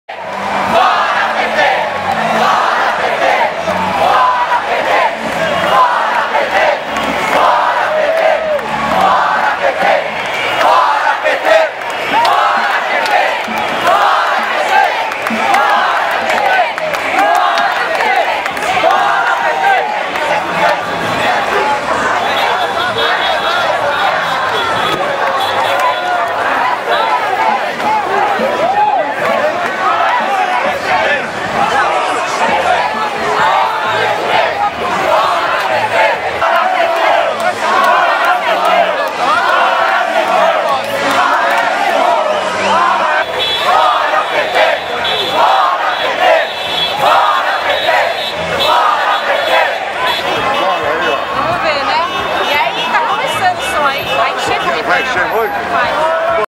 Rally for Aécio Neves in São Paulo, 25 October
Rally for PSDB Brazilian presidential candidate Aécio Neves on Av. Paulista, São Paulo, 25 October 2014.